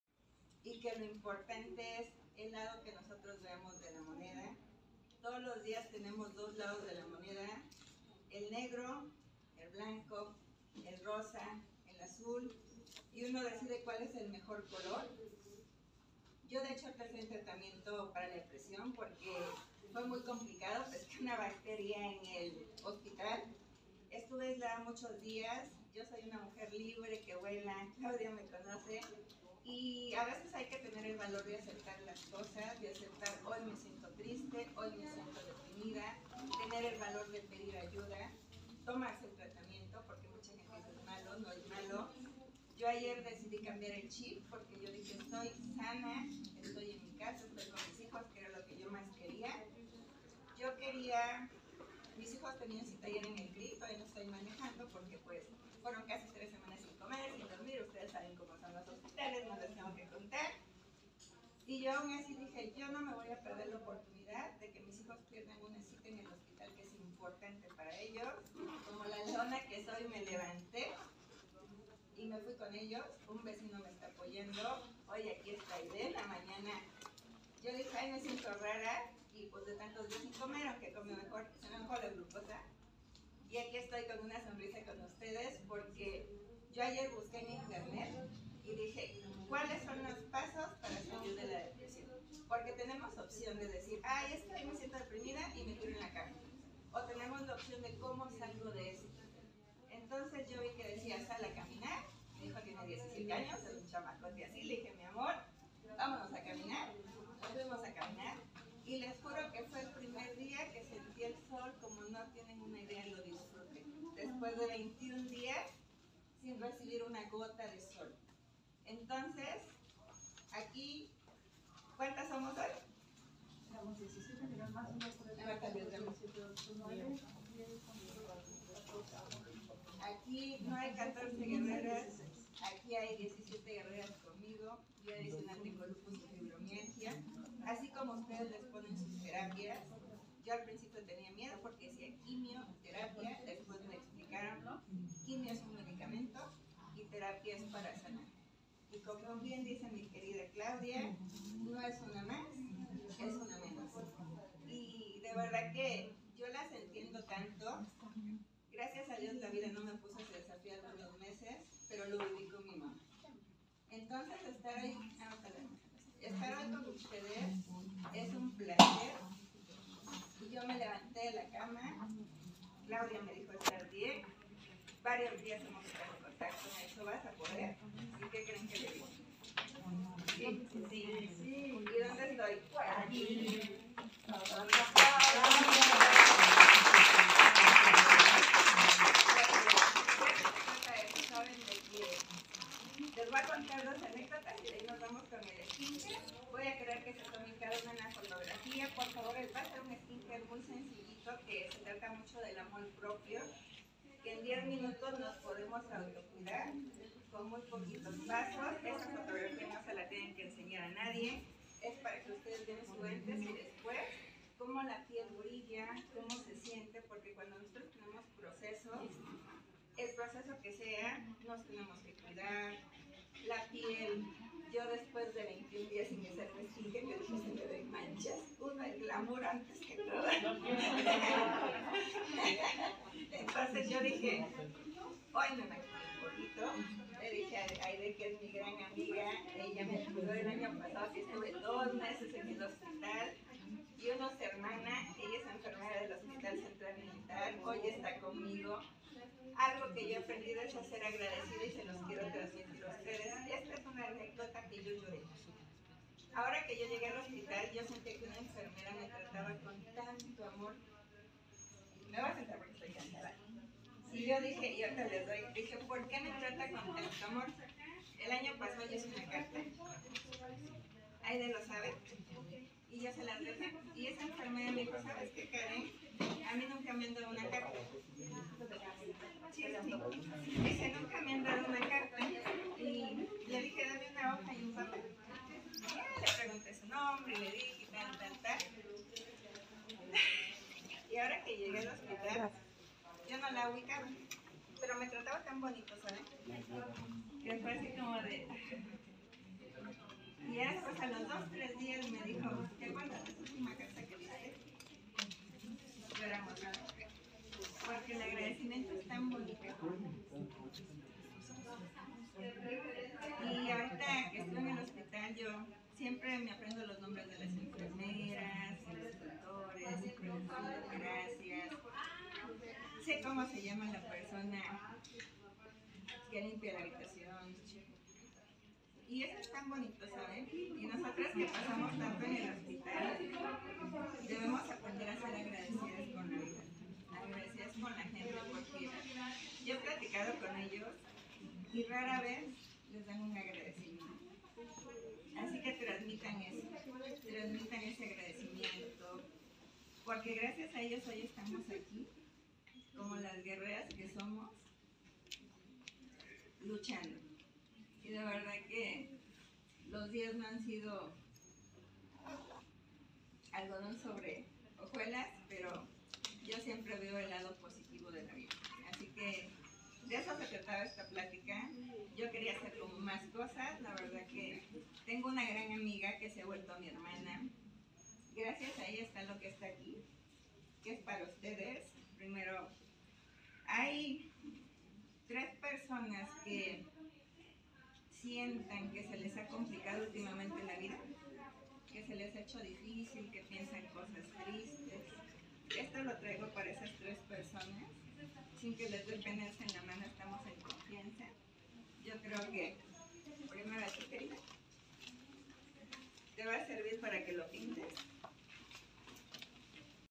Depresión y ansiedad
Así se ve la depresión

Este es un fragmento del curso que di en Octubre

lo más importante con tratamiento mejora y cada día es mejor que el anterior

la depresión no se ve esta es la tercera vez en  mi vida que me diagnostican con depresión

estar en el hospital 3 semanas y contagiarme del Clostridium dificille me afecto mentalmente

afortunadamente inicie con tratamiento y cada día me siento mejor adicional tomo terapia psicológica

como ven la depresión no impide vivir una vida normal

cuando sea el momento indicado dejaré los antidepresivos de forma paulatina con indicaciones de mi psiquiatra y lo más importante es que no causan adicion

La empatia es muy importante por que nadie sabe que lucha tienen las personas